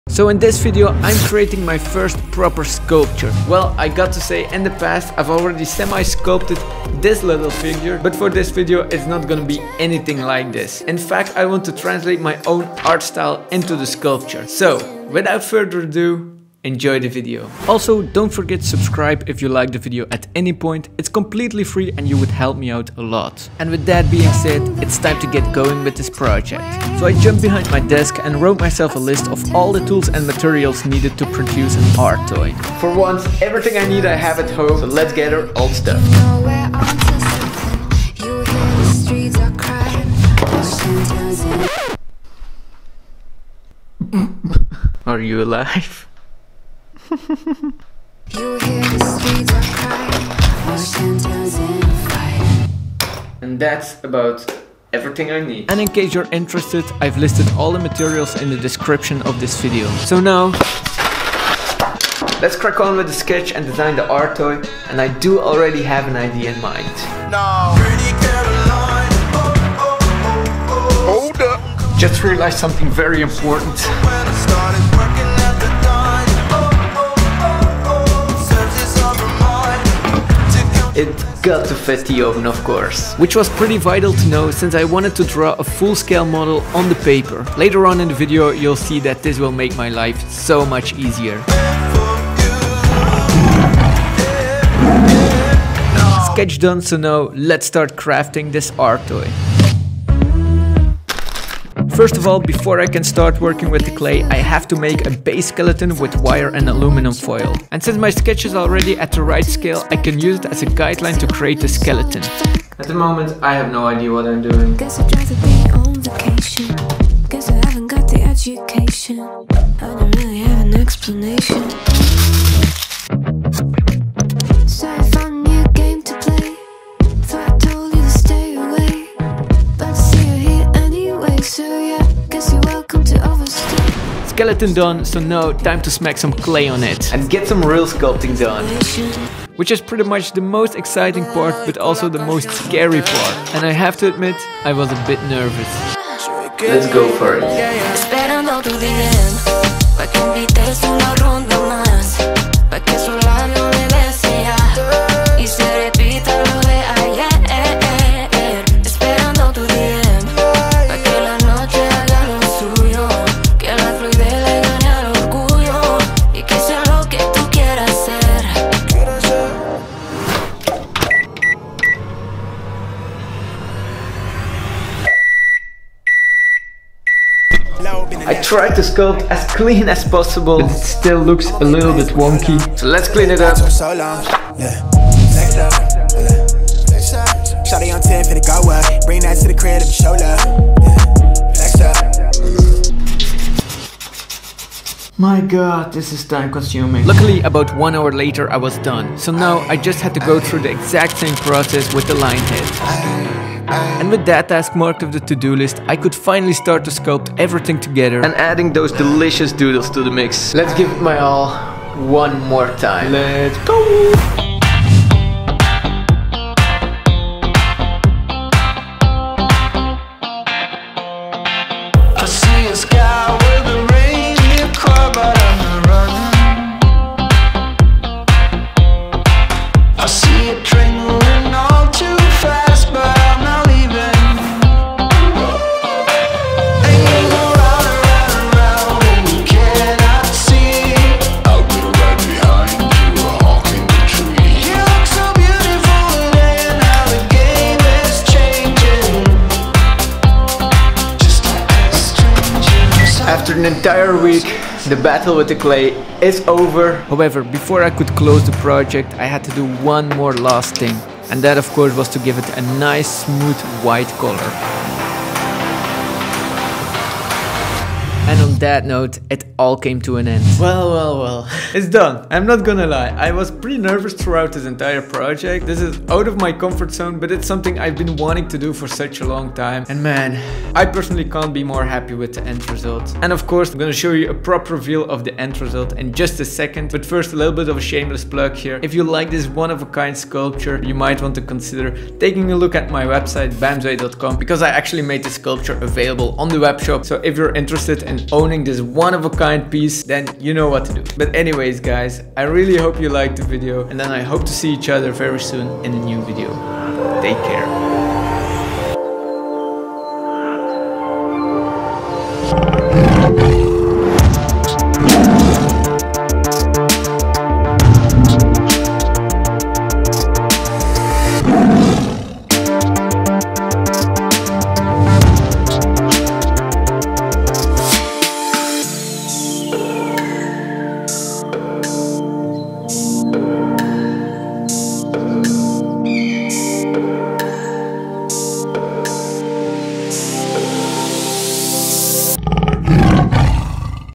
Sculpting an ART TOY like a PRO (full process)
In this video I make my own art designer toy from sketch to sculpt :) I hope you like it!

🎨 Tools & Materials used:
Polymer clay: Super Sculpey Beige, spray paint: Molotow Flame Blue, aluminium foil, sand paper, wire, tape, pliers & some basic sculpting tools

Thank you all so much for watching me sculpt my first art toy :) Let me know if you have any feedback on the video or the art itself, I'd love to hear about it!

See you all very soon with more art videos!
- BAM